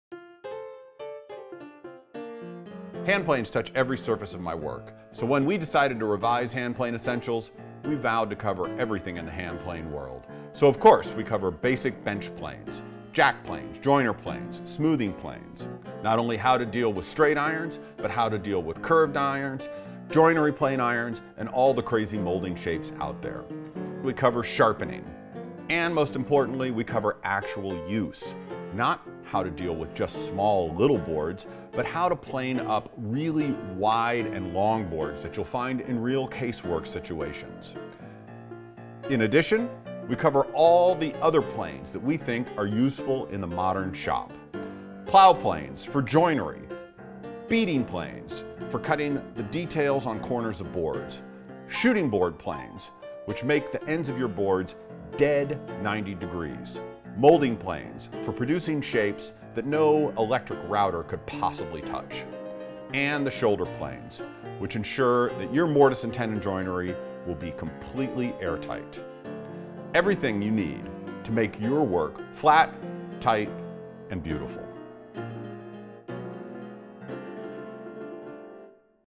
Handplane Essentials (Preview)
The revised and expanded compendium on everything hand planes from hand tool expert Christopher Schwarz. Learn about sharpening, maintenance, selecting the proper tool for the job, specialty planes and learn how to use each one.